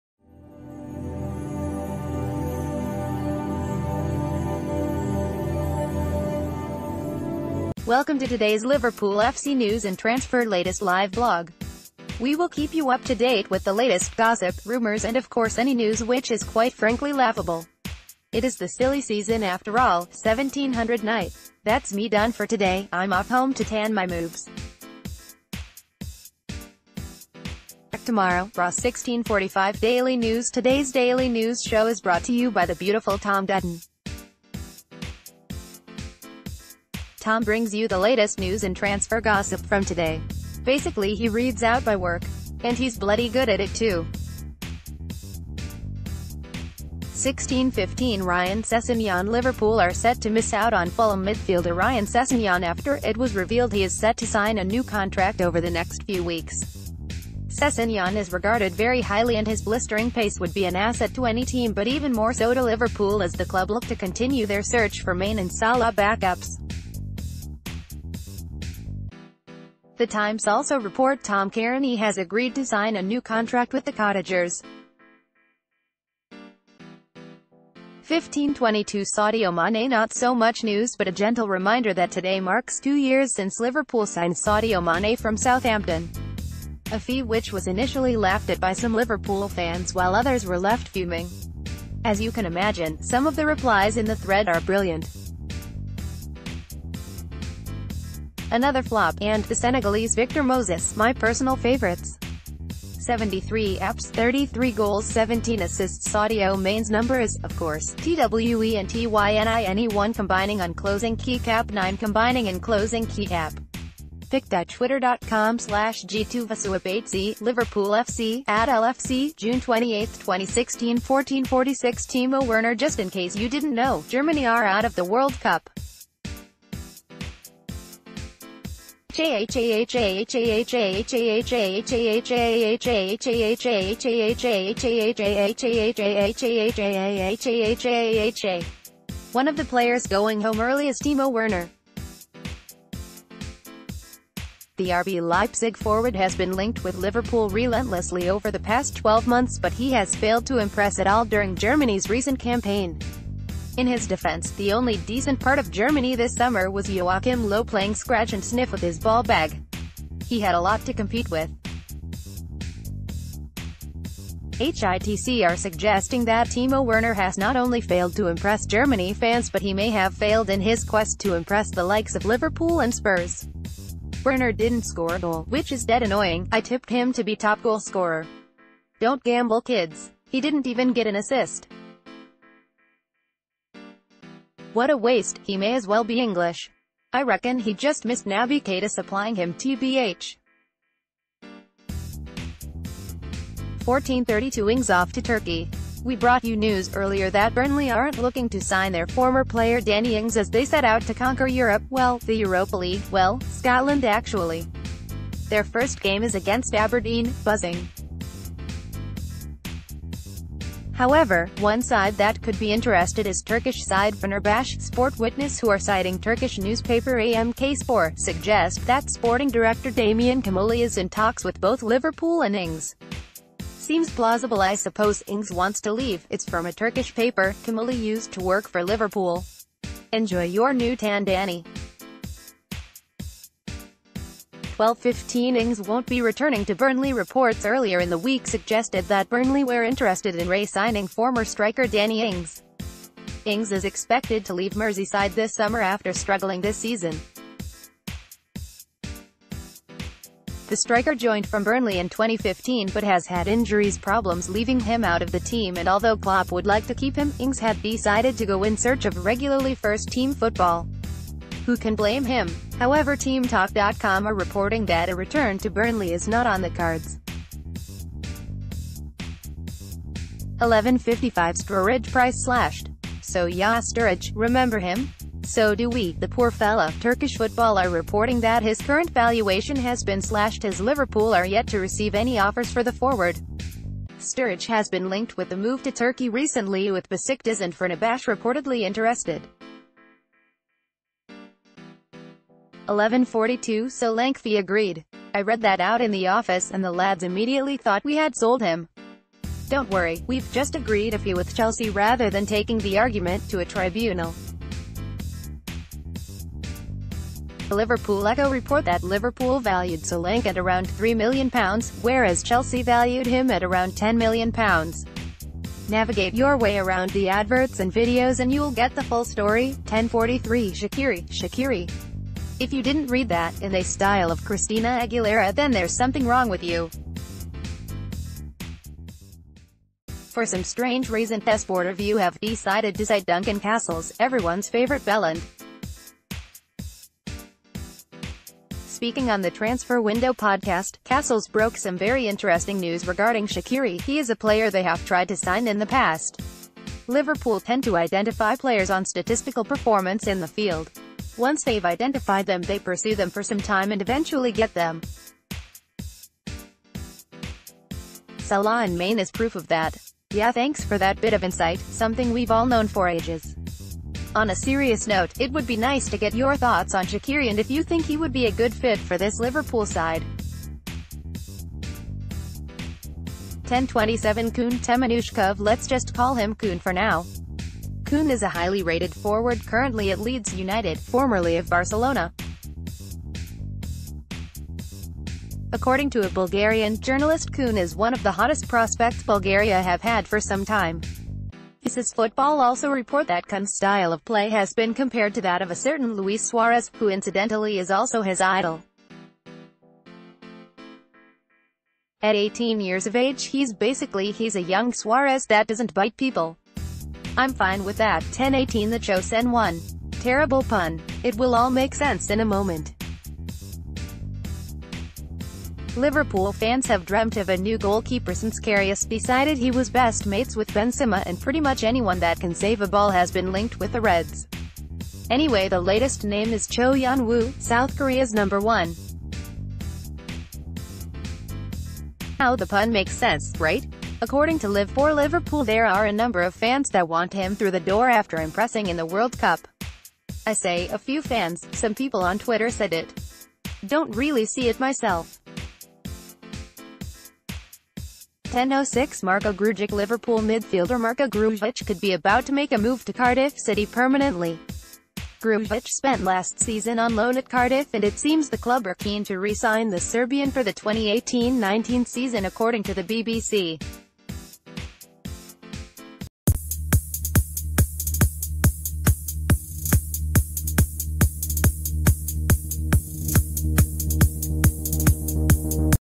Breaking News | Liverpool FC News & Transfer Latest | 28th June 2018
Welcome to today’s Liverpool FC news and transfer latest live blog! We will keep you up to date with the latest gossip, rumours and of course any news which is quite frankly laughable! It is the silly season after all!  17:00 – Night! Thats me done for today, I’m off home to tan my moobs. Tom brings you the latest news and transfer gossip from today. Basically he reads out my work. And he’...